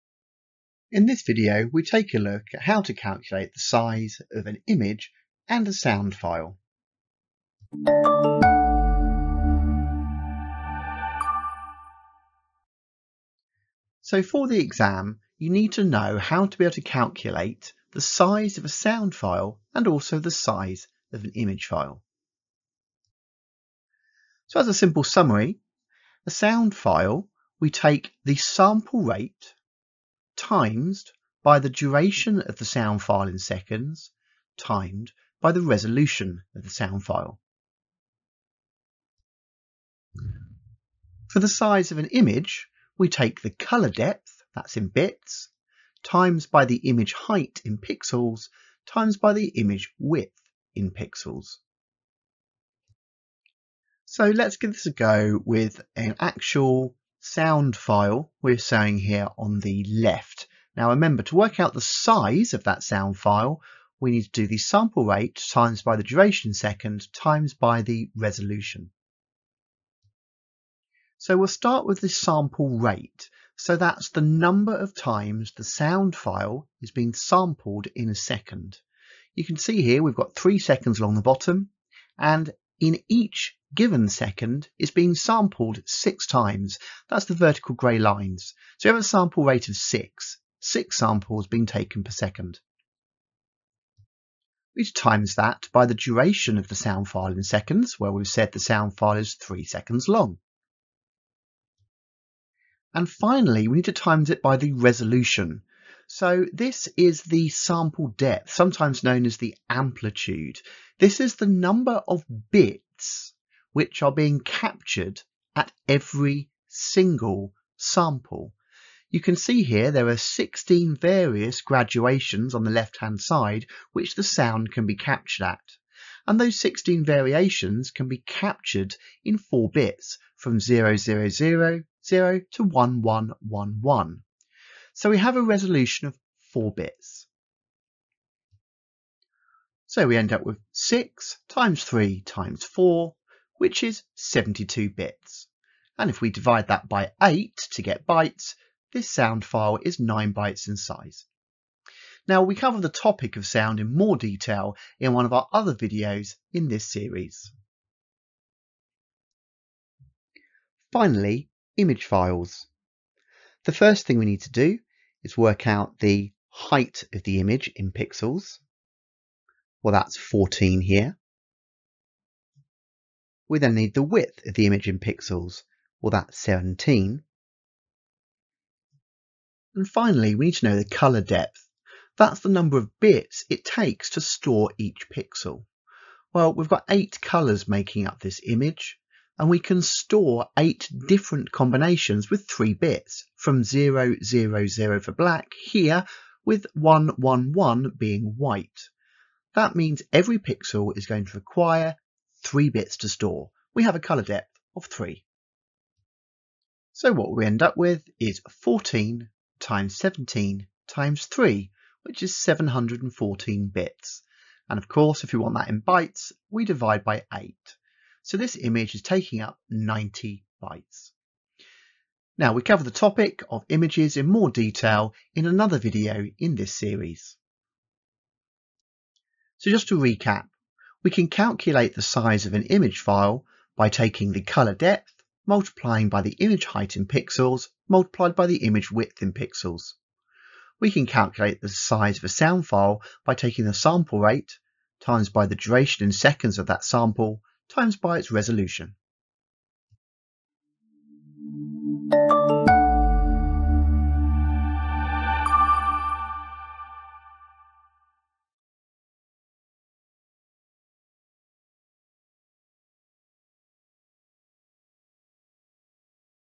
13. CAMBRIDGE IGCSE (0478-0984) 1.3 Calculating the size of an image and sound file
CAMBRIDGE 0478 & 0984 Specification Reference - Section 1.3 - 2

00:00 Calculating the size of an image and sound file
00:08 Intro
00:13 Calculating file sizes
00:53 Calculating sound file sizes
02:39 Calculating image file sizes
03:49 Recap
04:10 Outro